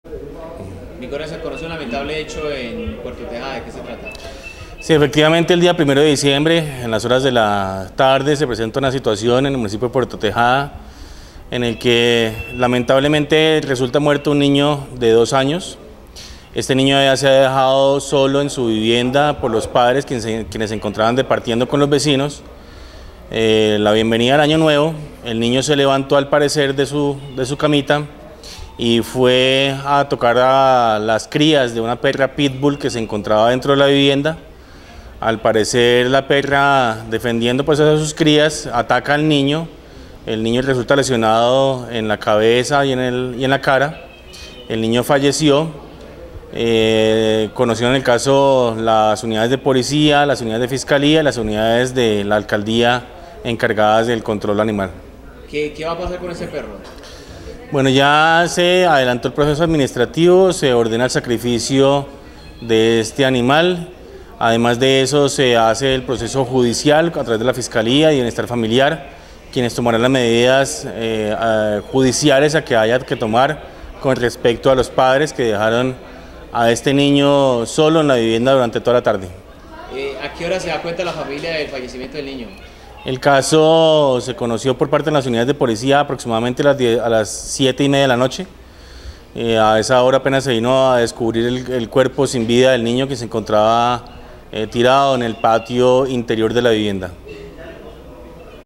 Perra Pitbull mató ayer a un niño en Puerto Tejada
Un niño de aproximadamente 3 años de edad se encontraba durmiendo en su cama mientras sus padres departían con sus vecinos en el antejardín de su casa.

El niño despierta y nota que no hay gente en la casa y se pone a observar su perra Pitbull, la que en días pasados había dado cría, el niño desea jugar con los cachorros, se acerca a ellos y es en ese momento en el que es atacado en varias partes del cuerpo por el animal, heridas que le causan la muerte.